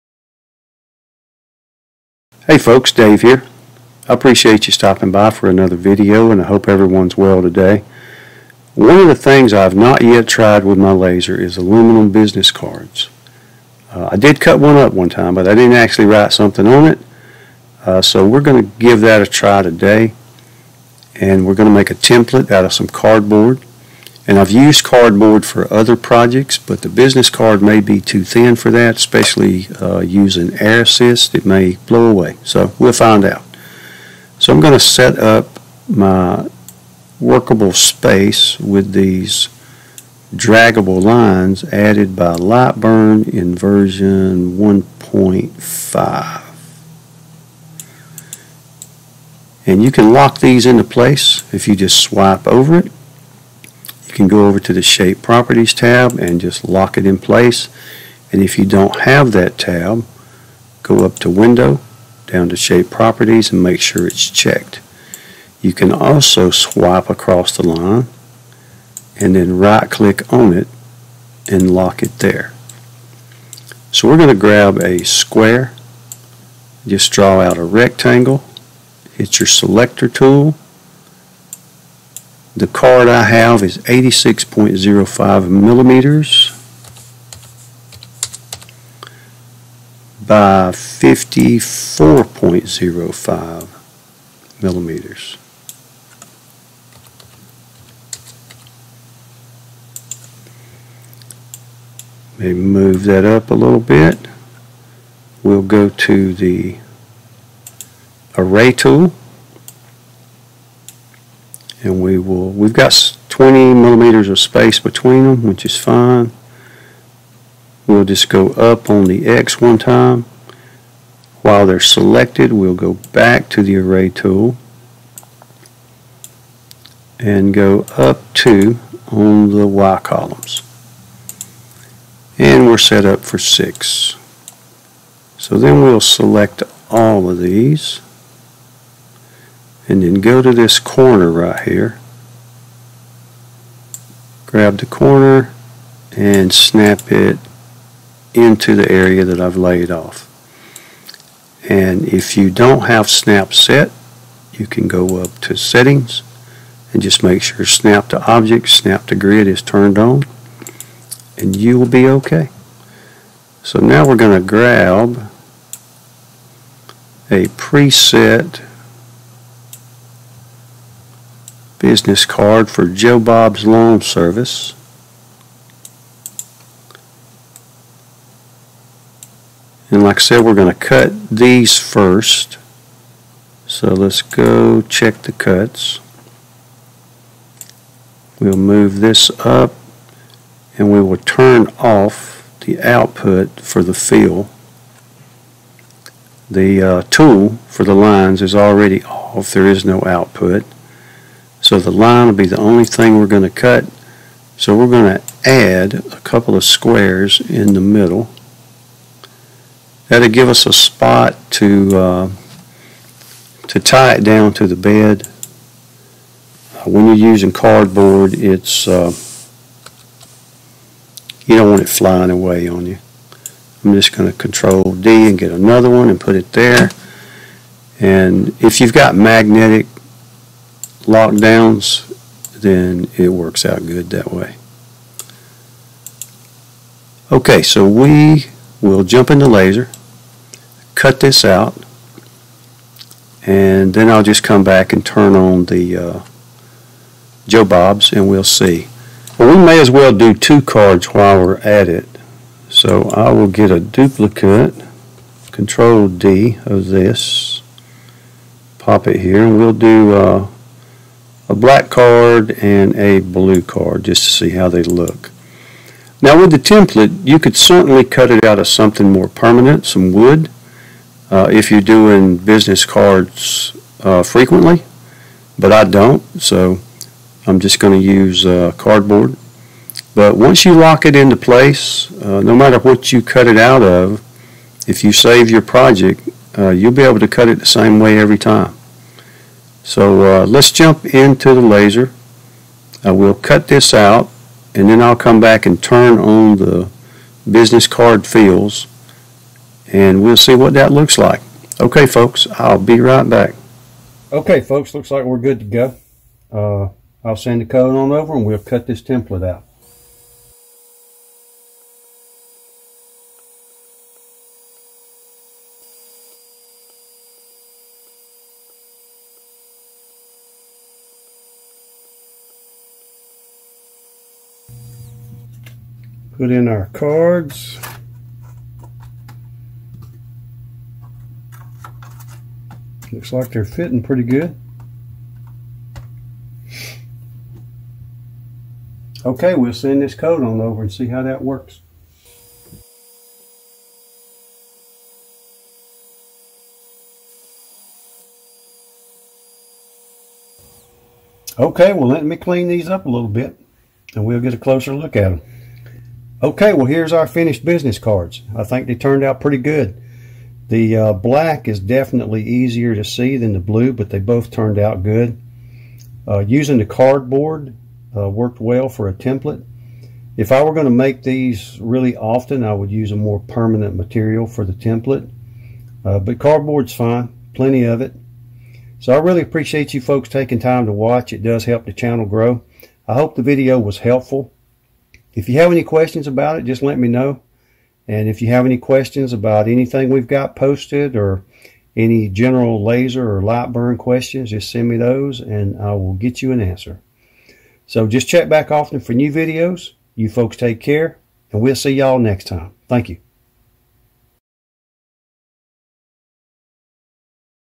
Laser Engrave Anodized Aluminum | Beginner Laser Project #47
This video describes and demonstrates the step-by-step laser engraving anodized aluminum business cards with Lightburn laser cutting software and the XTOOL D1 Pro 40W diode laser.

Chapters:
0:00 Opening remarks
0:08 Project overview
0:33 Lightburn step-by-step
5:03 Laser cutting template
5:26 Laser engraving business cards
5:55 Project review